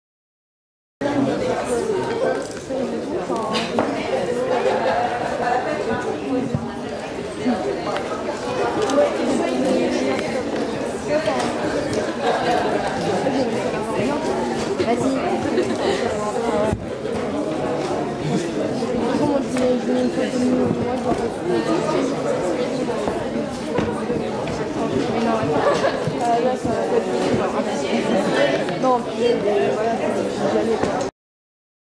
DANIEL RADCLIFFE : LES AUTOGRAPHES
Après la conférence de presse du  film IMPERIUM, l'acteur Daniel Radcliffe s'est prêté au jeu des autographes. Et ils étaient nombreux...le pauvre :)